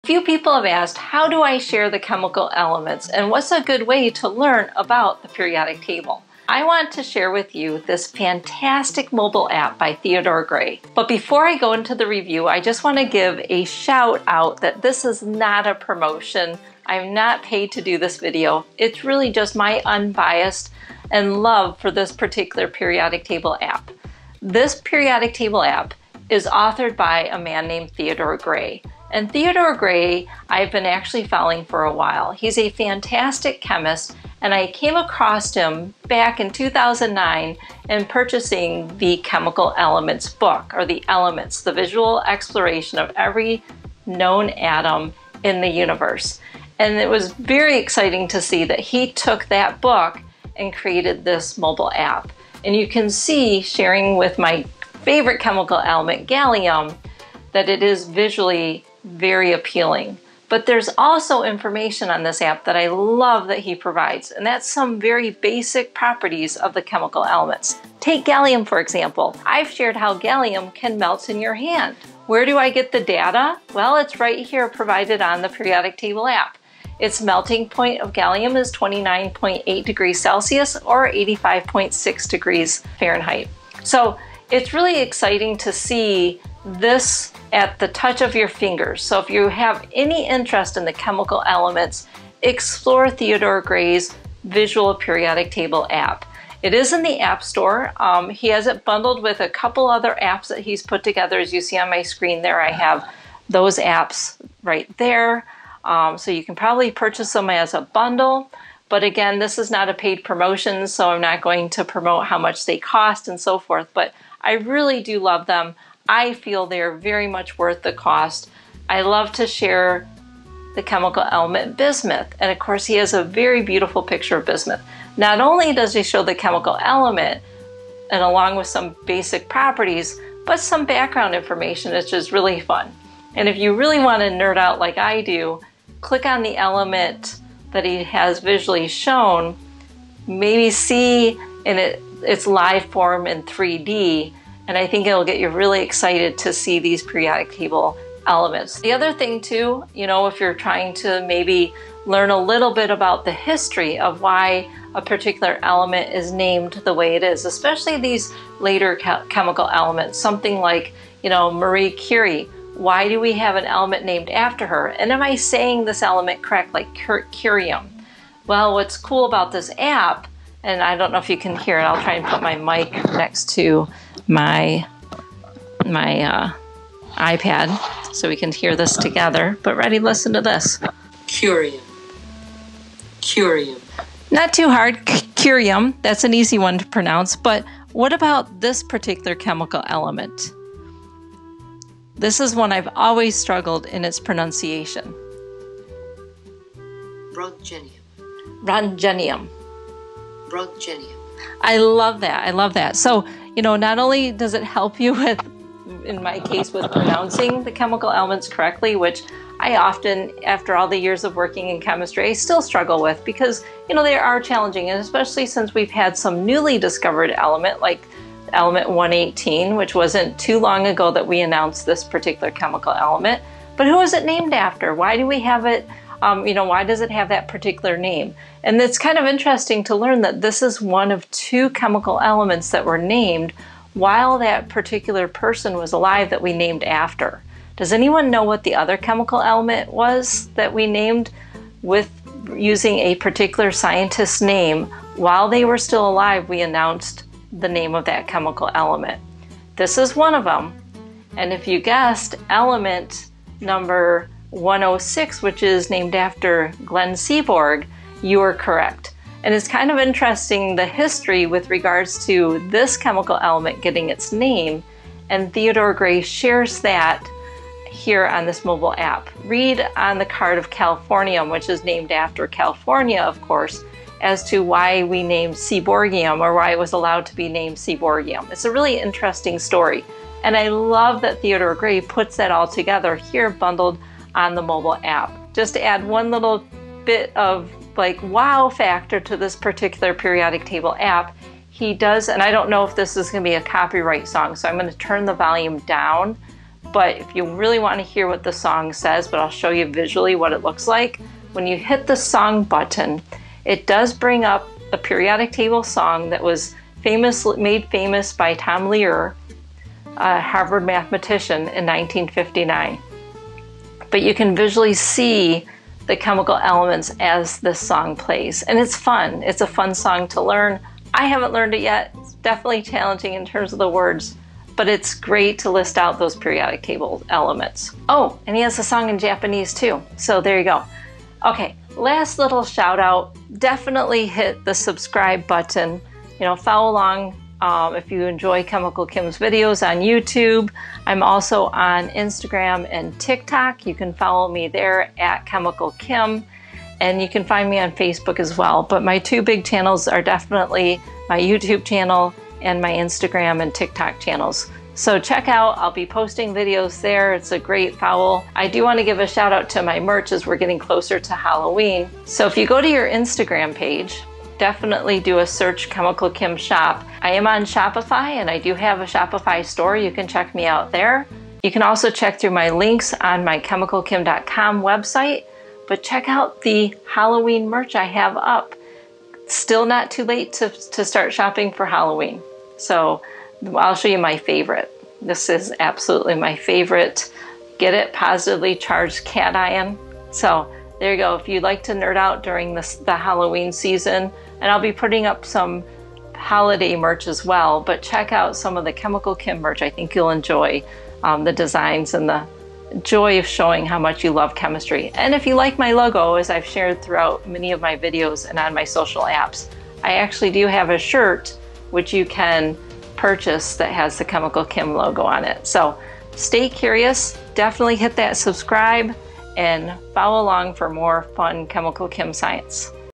My Favorite Periodic Table App Review
I have been a huge fan of Theodore Gray’s work and wanted to share a great way to learn about the periodic table.